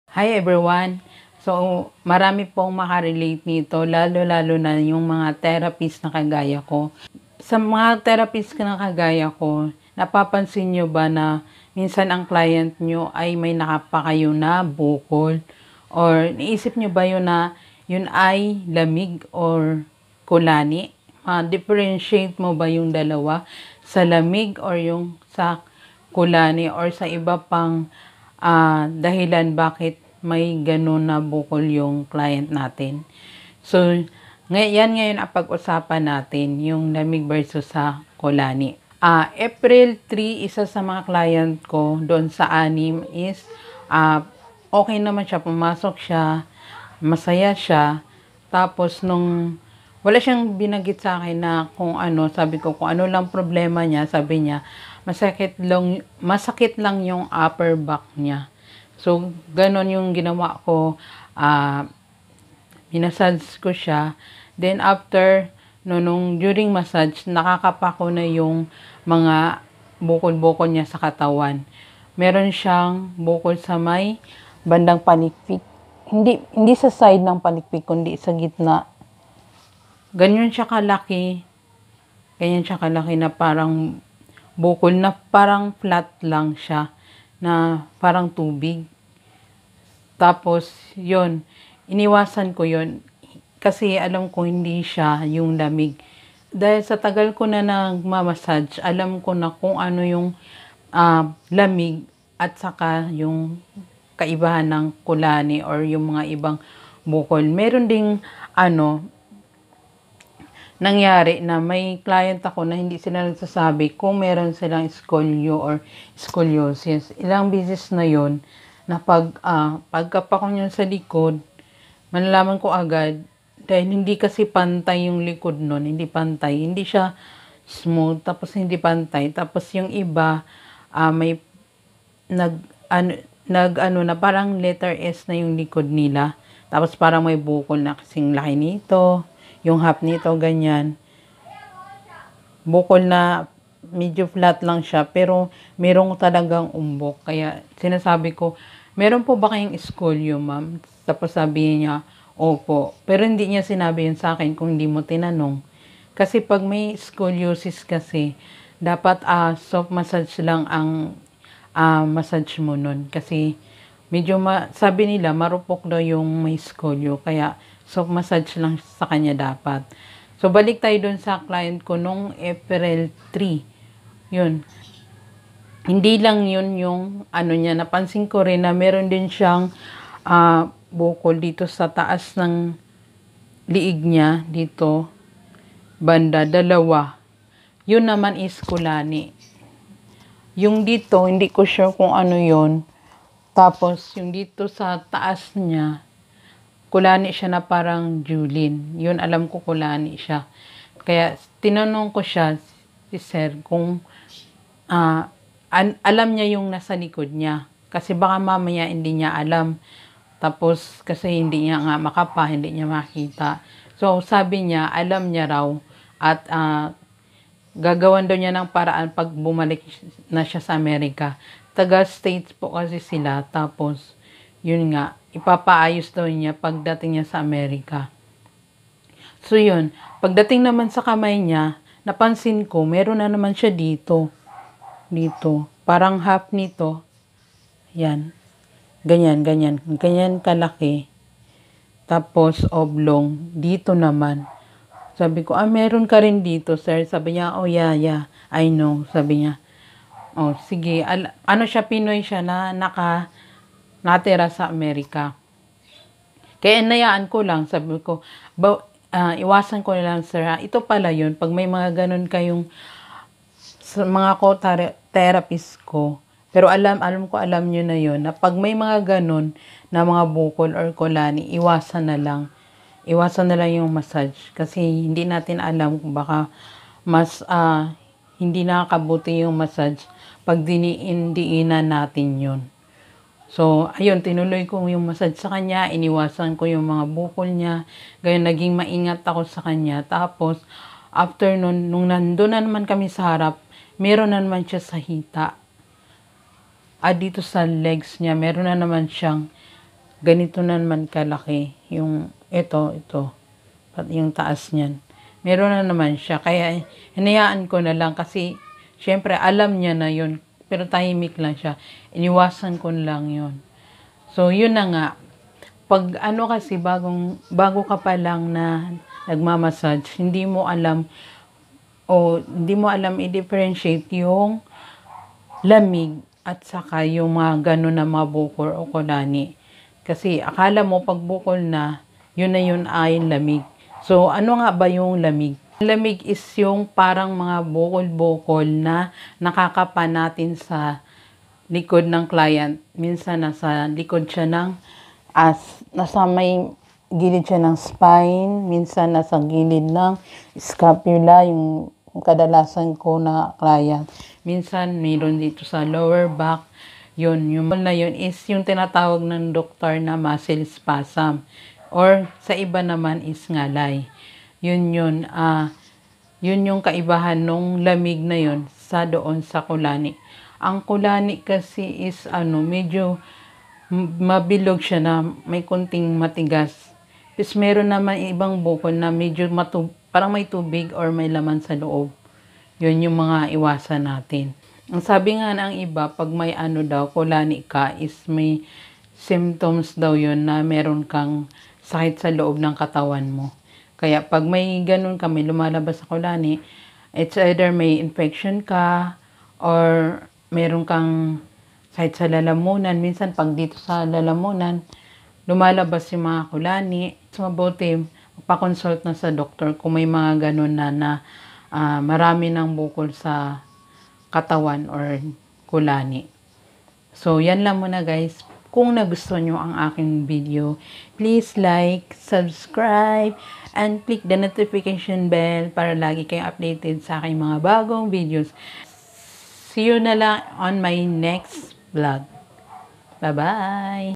Lahat ba ng Bukol Bukol sa katawan ay Lamig?
Hi everyone, maganda ang topic natin ngayon,it's about bukol bukol na nakakapa naming mga therapist ay matatawag ba na lamig ang lahat?